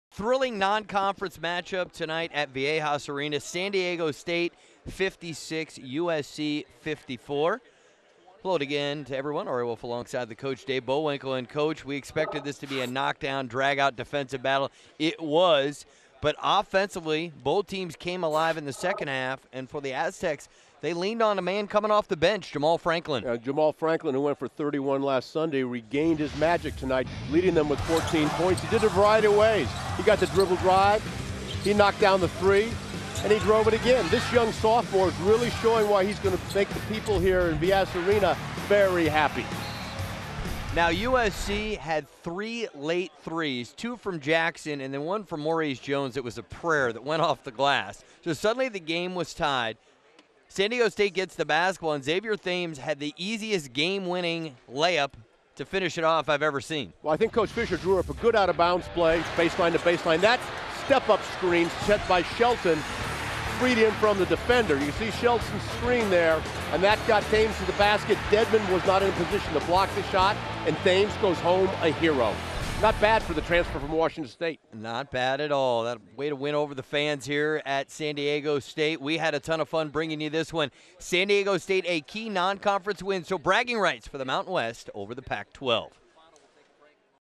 USC Trojans at SDSU Aztecs - NCAA Basketball 2011
Game played Nov. 2011 at Viejas Arena on San Diego State University campus.   Game televised by The MTN. (The MountainWest Sports Network)   carried nationwide on DirecTV Channel 616 and leading cable TV providers.